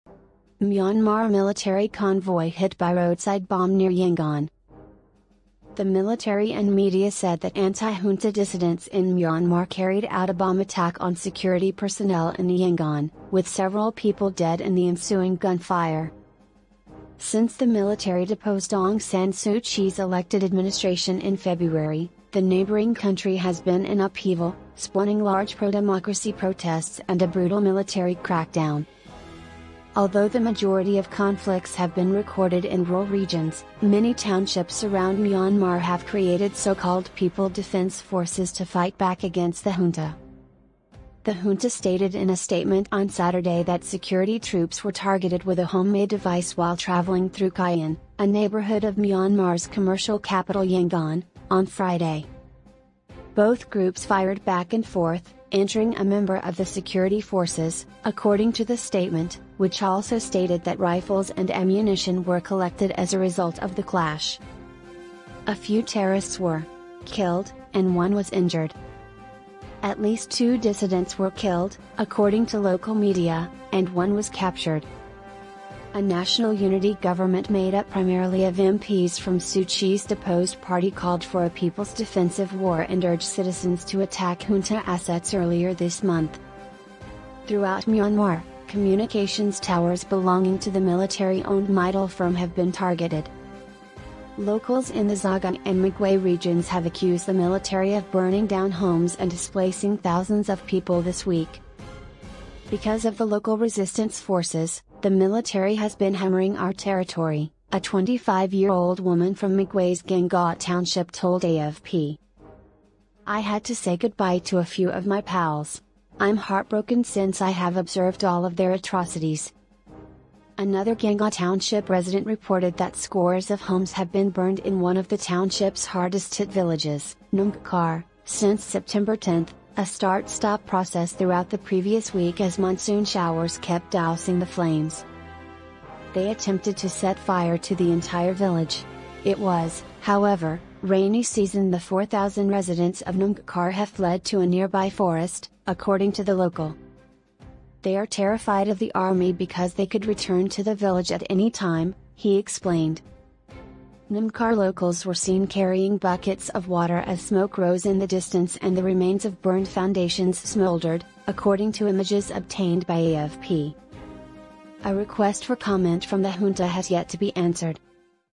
Myanmar military convoy hit by roadside bomb near Yangon
The military and media said that anti-junta dissidents in Myanmar carried out a bomb attack on security personnel in Yangon, with several people dead in the ensuing gunfire.

Since the military deposed Aung San Suu Kyi's elected administration in February, the neighboring country has been in upheaval, spawning large pro-democracy protests and a brutal military crackdown.

Although the majority of conflicts have been recorded in rural regions, many townships around Myanmar have created so-called "people defense forces" to fight back against the junta.

The junta stated in a statement on Saturday that security troops were targeted with a homemade device while traveling through Khayan, a neighborhood of Myanmar's commercial capital Yangon, on Friday.

"Both groups fired back and forth, injuring a member of the security forces,", according to the statement, which also stated that rifles and ammunition were collected as a result of the clash.

A few terrorists were... (killed), and one was injured.

At least two dissidents were killed, according to local media, and one was captured.

A "National Unity Government" made up primarily of MPs from Suu Kyi's deposed party called for a "people's defensive war" and urged citizens to attack junta assets earlier this month.

Throughout Myanmar, communications towers belonging to the military-owned Mytel firm have been targeted.

Locals in the Sagaing and Magway regions have accused the military of burning down homes and displacing thousands of people this week.

"Because of the local resistance forces, the military has been hammering our territory,", a 25-year-old woman from Magway's Gangaw township told AFP.

I had to say goodbye to a few of my pals... I'm heartbroken since I have observed all of their atrocities.

Another Gangaw township resident reported that scores of homes have been burned in one of the township's hardest-hit villages, Namg Kar, since September 10 — a start-stop process throughout the previous week as monsoon showers kept dousing the flames.

"They attempted to set fire to the entire village. It was, however, rainy season ", The 4,000 residents of Namg Kar have fled to a nearby forest, according to the local.

"They are terrified of the army because they could return to the village at any time," he explained.

Namg Kar locals were seen carrying buckets of water as smoke rose in the distance and the remains of burned foundations smouldered, according to images obtained by AFP.

A request for comment from the junta has yet to be answered.